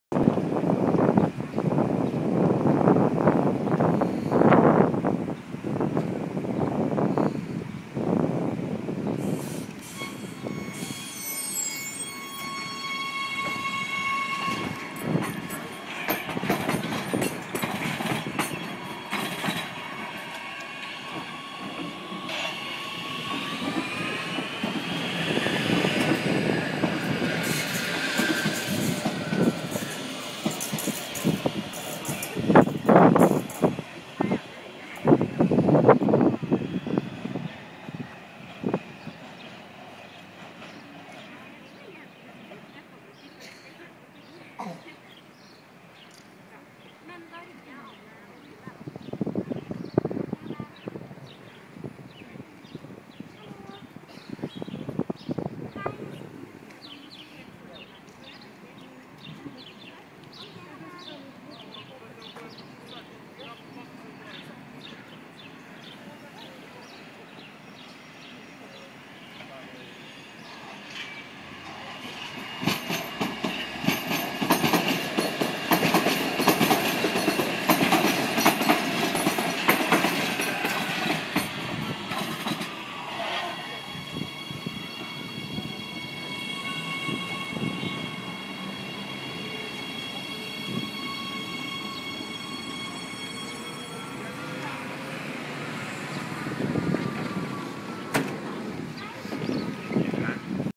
Oslo T-bane action at Majorstuen station.
1. Line 1 train from Helsfyr to Holmenkollen and Frognerseteren departs.
2. Line 5 train from Storo to Vestli arrives.
3. Line 5 train from Vestli to Storo arrives.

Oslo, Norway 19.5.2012